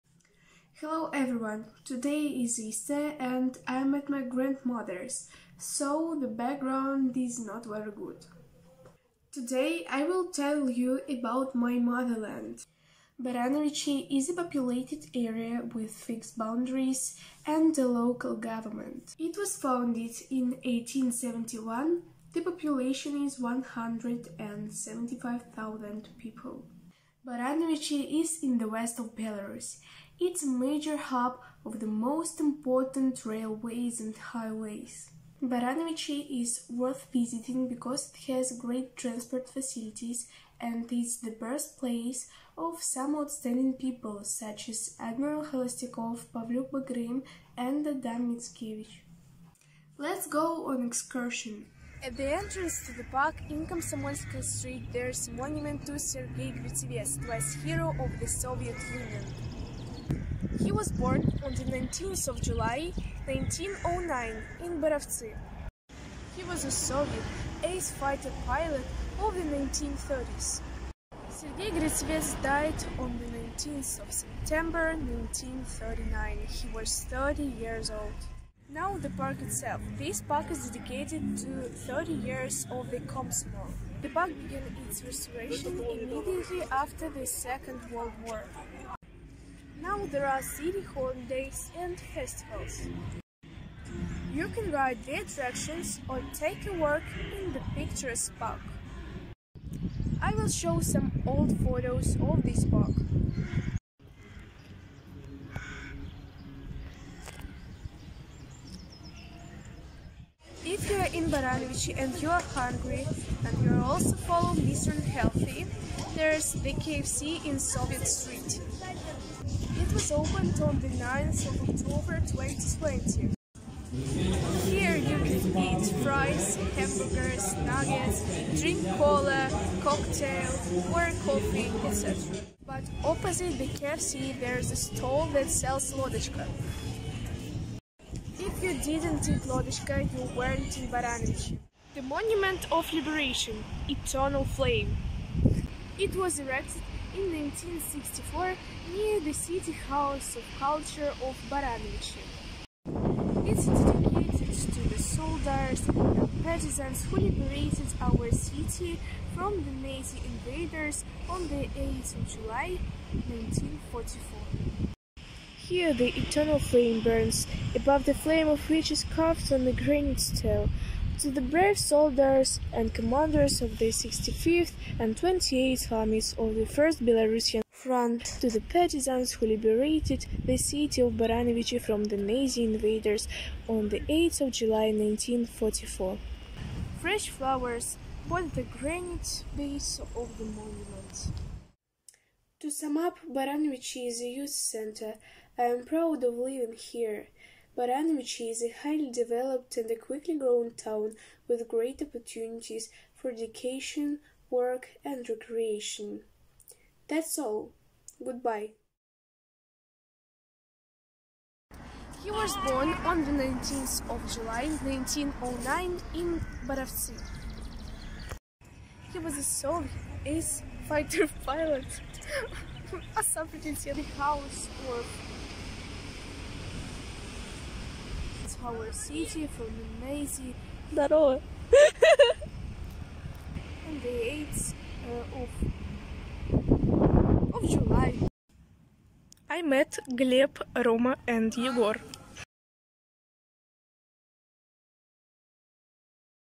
A virtual guided tour around my hometown! Happy 150th birthday, Baranovichi! -Alesya Morgun
This video created by Alesya Morgun  of 7'E1' grade will tell you about the most breathtaking places in Baranovichi'. This video is dedicated to Baranovichi's 150th birthday! We wish everyone good mood and delightful moments on this pleasant holiday!